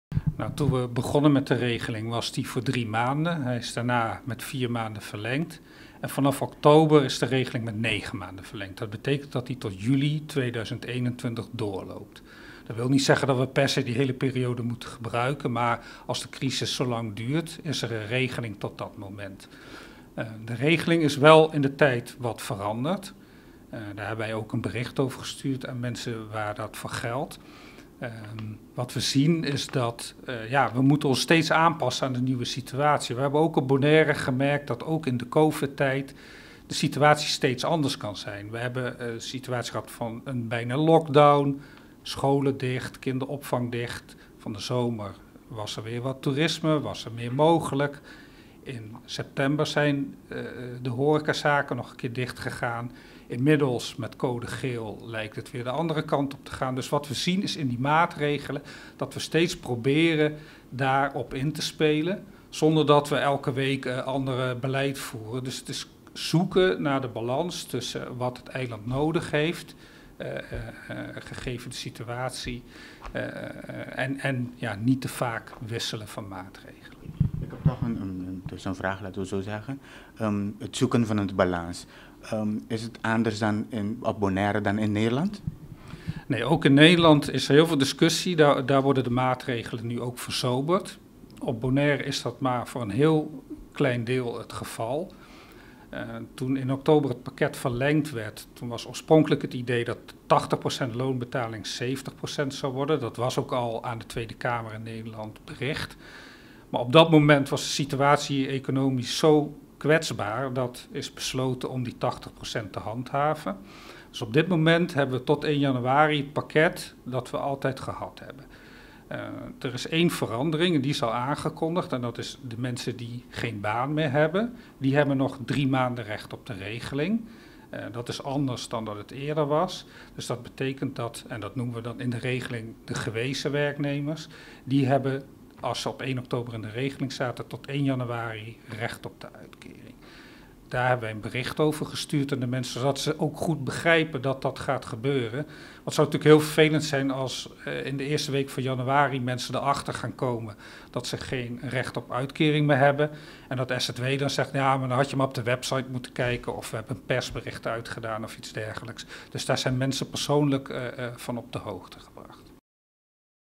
Eric Brakke SZW 1 PAP
Notisia NosTv a kombersa ku sr Erick Brake hefe di departamentu di asuntunan sosial i labor ku a splika nos detaye di e areglo di pago ku a keda adapta.